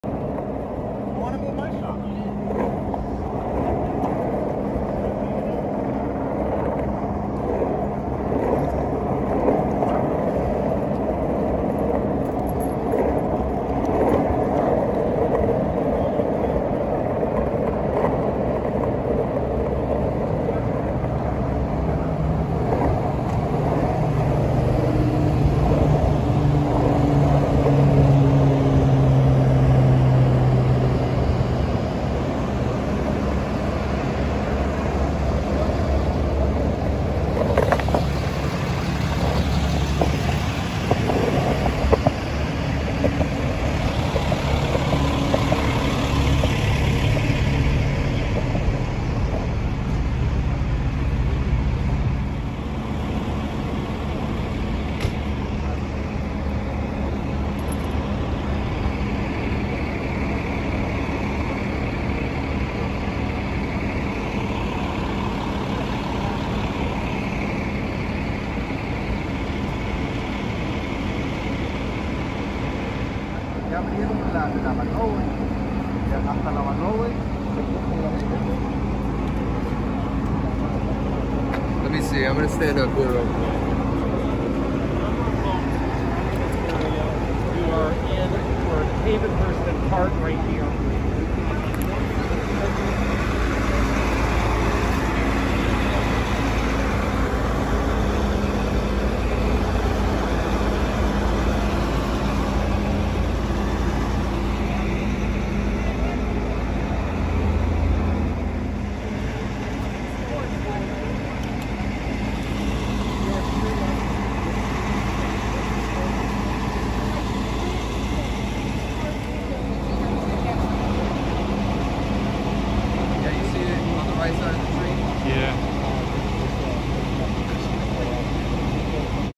Airplane Crash Near Van Nuys Airport On 9/11
An airplane crashed today next to the Van Nuys airport on September 11, 2020